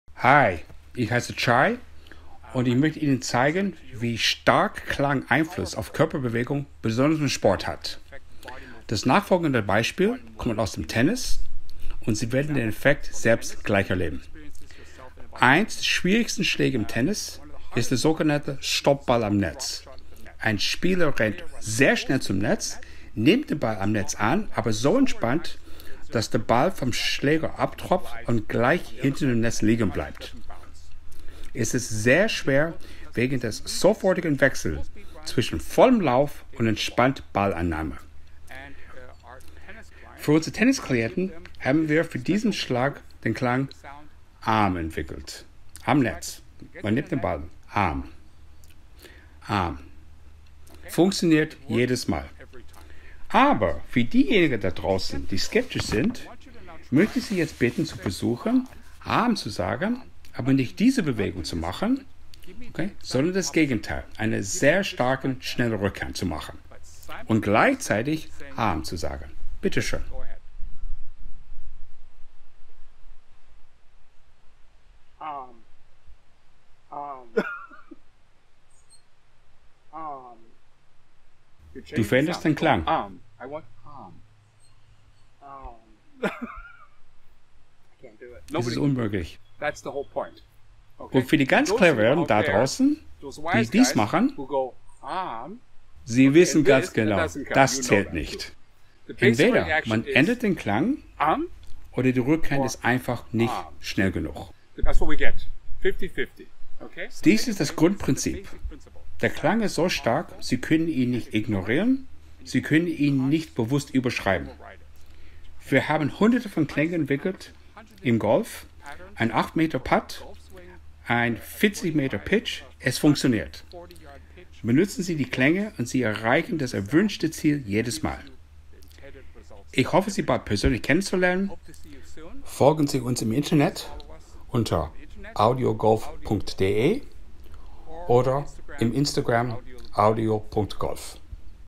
Audio Golf - Klang und Körperbewegung
Für die Meisten ist es schwer vorstellbar, wie Klänge die Golfschwungbewegungen beeinflussen. Hierfür haben wir ein sehr einfaches Beispiel, das jeder selbst ausprobieren kann.

Probier es aus und du wirst schnell merken, wie effektiv unsere Methode ist und warum wir jeden Schwung in wenigen Minuten verbessern können.